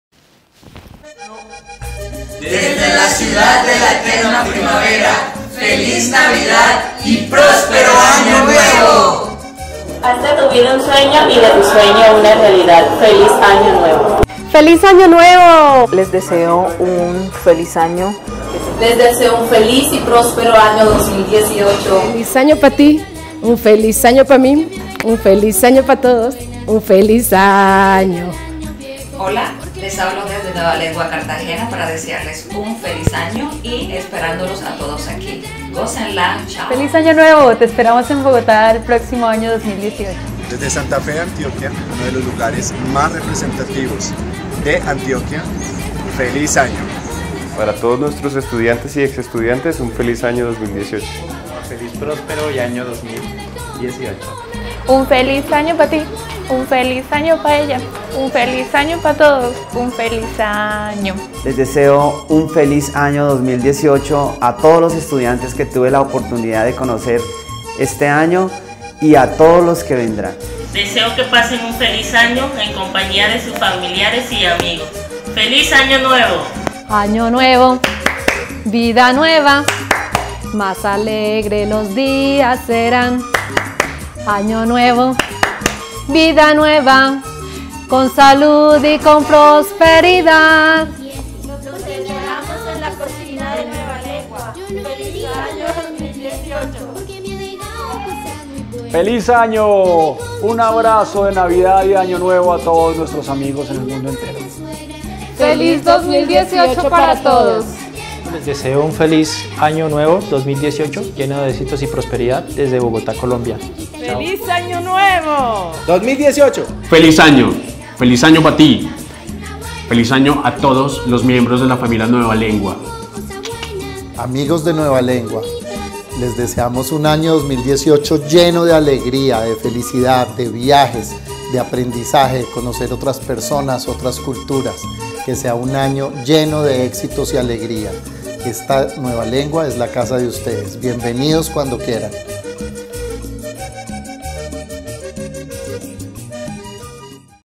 ¡Feliz año desde Nueva Lengua!
A todos nuestros estudiantes: presentes, pasados y futuros ¡feliz 2018!
Los esperamos para estudiar español en Colombia